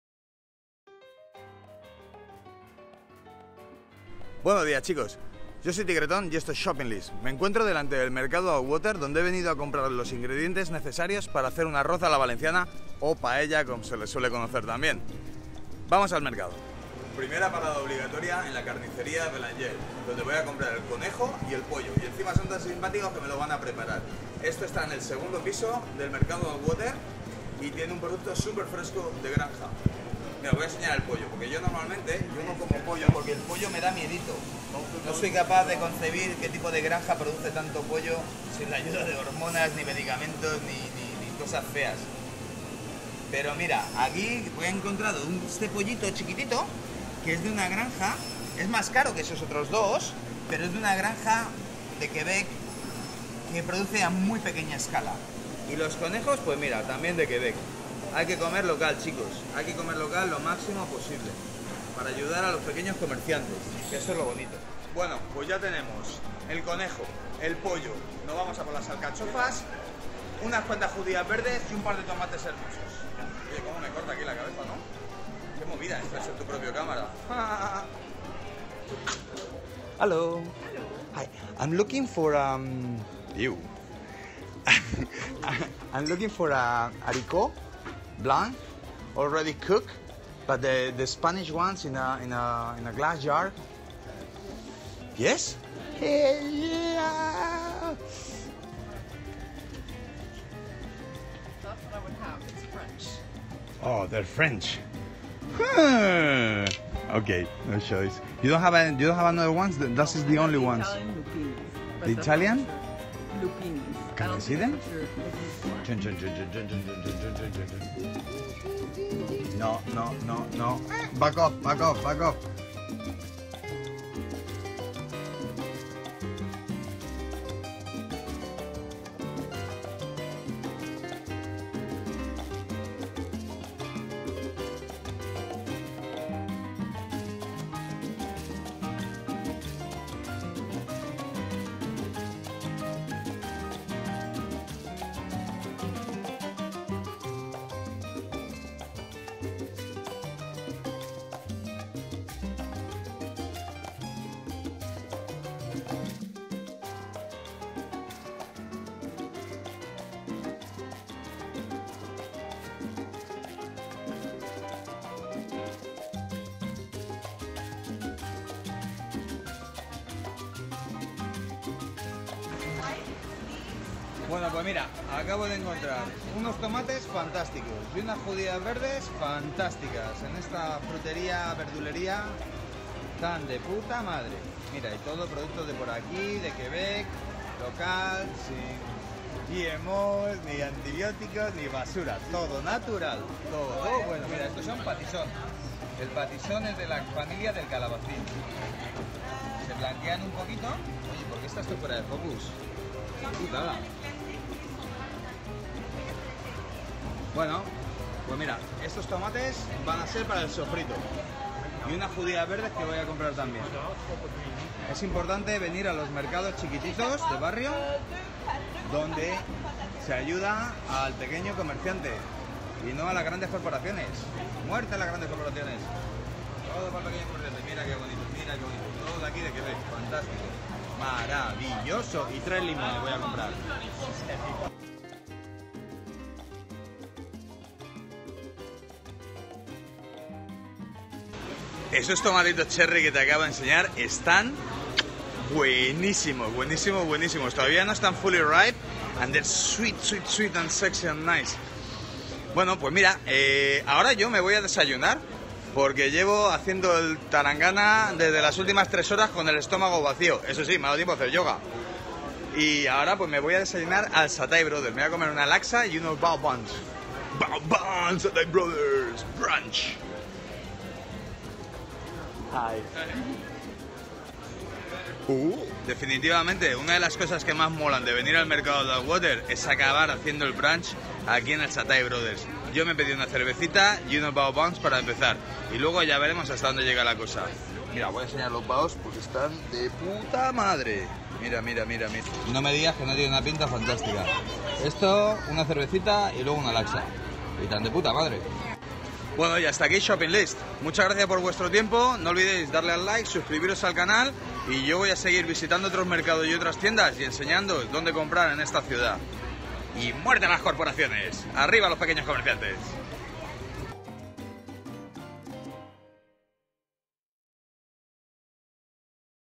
Shopping list 03, donde comprar pollo y conejo en Montreal, de confianza?
Esto es Shopping List, un espacio en La Mesa de Tigre donde compartiré con vosotros mis secretos mejor guardados. Donde comprar en Montreal el mejor género, al mejor precio.  Porque recuerda que somos lo que comemos.

No sé a vosotros, pero a mí se me hace particularmente complicado comer pollo. No concibo qué tipo de granja puede producir tal cantidad de pollo si la ayuda de hormonas y antibióticos, por lo cual se ha convertido en la fuente de proteína a la que suelo mirar con recelo. Me he ido hasta el mercado de Atwater Donde sé que hay una carnicería que vende unos pollitos fantásticos de granja, pequeña producción como mandan los cánones!!...Y de paso me he marcado un brunch de puta madre en los Satay Brothers, life is good :)

Muchas gracias por compartir vuestro tiempo conmigo.

Se os quiere.